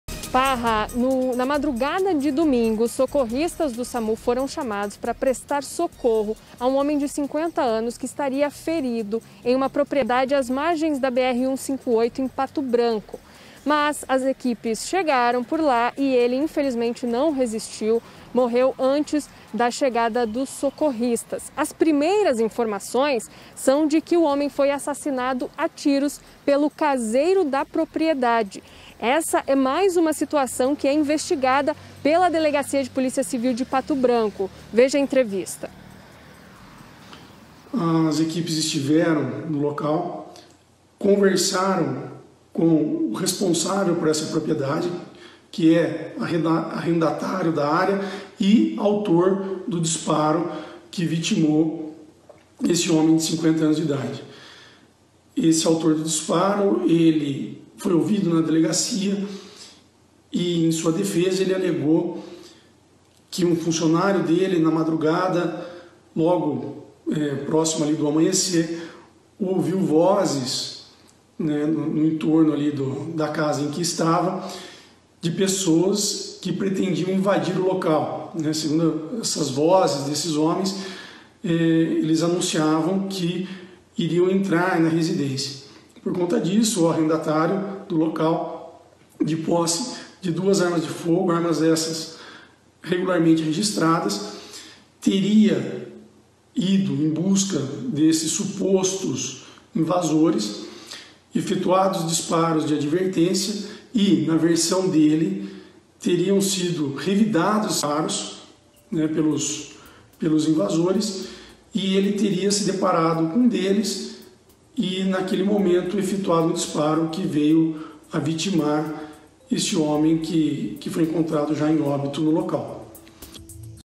Homem é morto após invadir propriedade rural
Um homem foi morto na madrugada deste domingo (15), em uma propriedade rural localizada na BR-158 nas proximidades do bairro Planalto, em Pato Branco. De acordo com informações levantadas pelo PB Urgente, o homem que não teve o nome divulgado foi morto pelo caseiro. Confira todos os detalhes na reportagem!